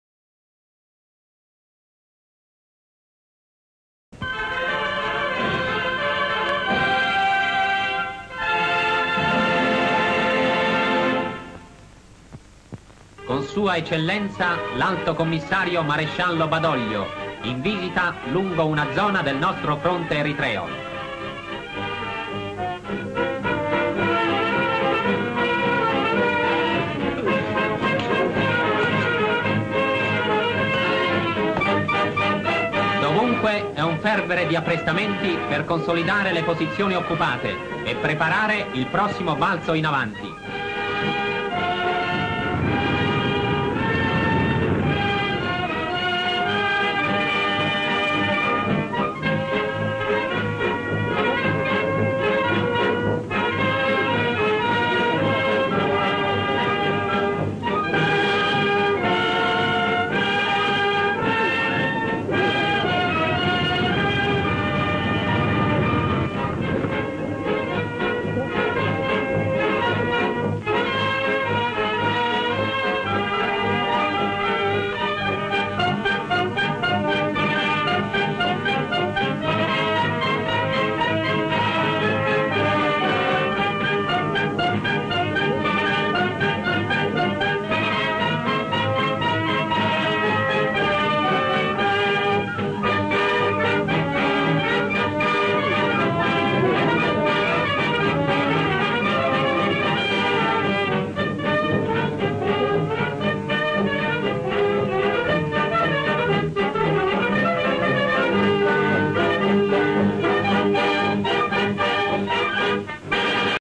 Il maresciallo Badoglio visita le truppe italiane sul fronte eritreo.
Descrizione sequenze:visita del maresciallo Badoglio ad alcune truppe italiane sul fronte eritreo ; alcuni soldati al lavoro ; operazioni militari di consolidamento delle postazioni delle nostre truppe sul fronte ; spostamenti delle nostre truppe ; il maresciallo Badoglio viene edotto sull'andamento delle operazioni militari ; panoramiche sulla zona del fronte interessata dalle operazioni ; il maresciallo Badoglio prosegue la visita ; Badoglio scruta col binocolo l'orizzonte in lontananza ;